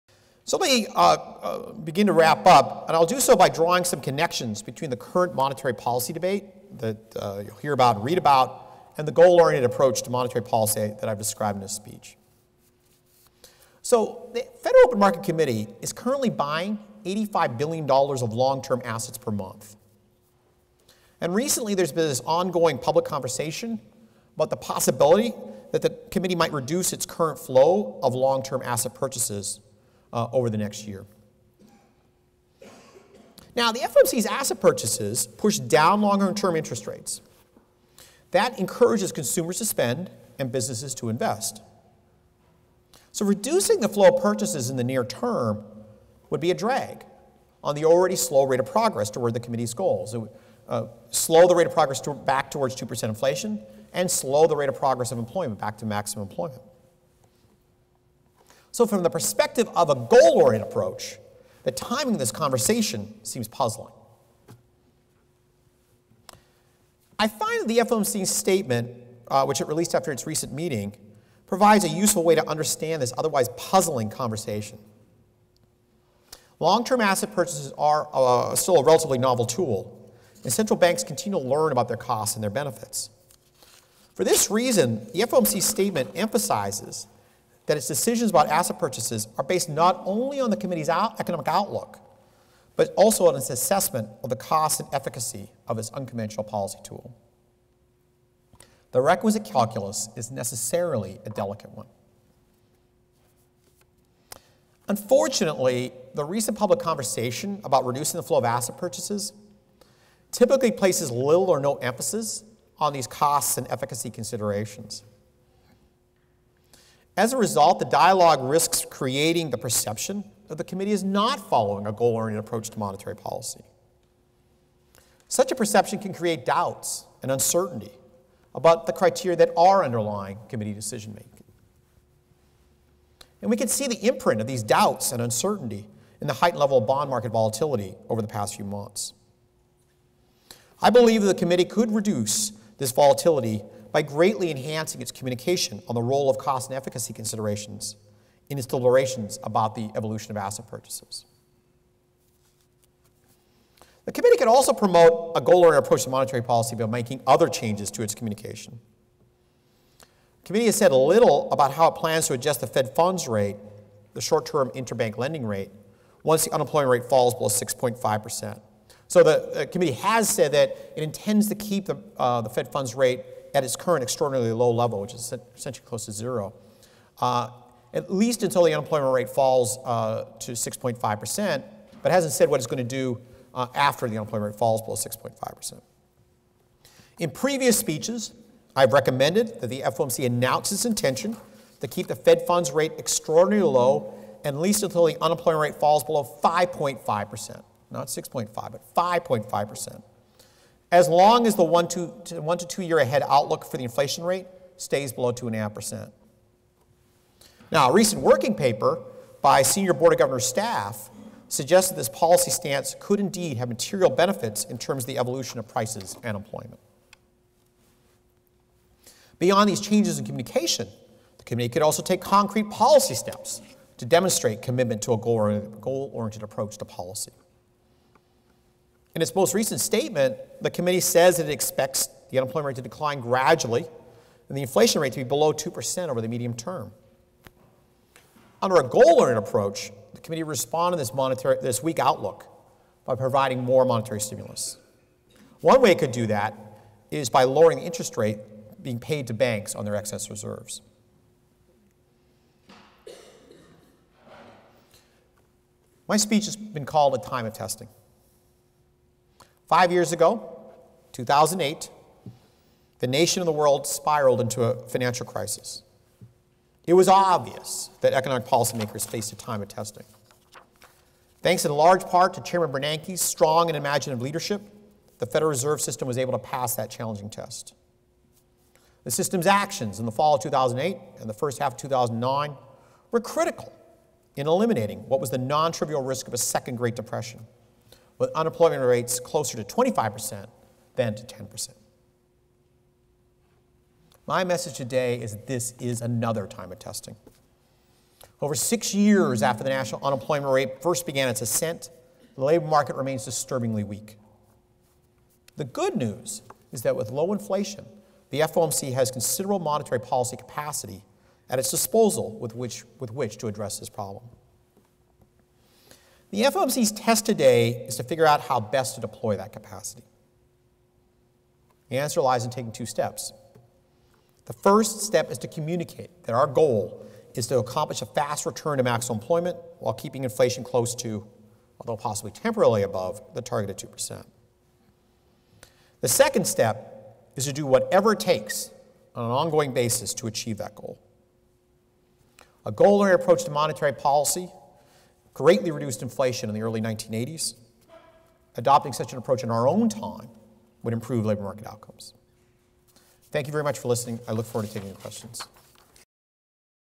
Kocherlakota Speech (excerpt): "Communication Changes and Policy Choices."
Narayana Kocherlakota, President of the Federal Reserve Bank of Minneapolis, addresses the St. Paul Area Chamber of Commerce on November 12, 2013. These remarks are an update to his complete speech, "A Time of Testing," presented in Houghton, Michigan September 26, 2013